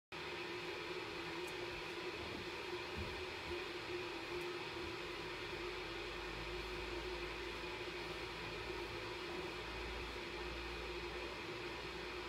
Ladies Joya Kyoto Rocker Sole Sandal
See what the Ladies Joya Kyoto Sandal looks like. Watch as Kate from Cheerful Soles tries this very bling rocker sole sandal on for you.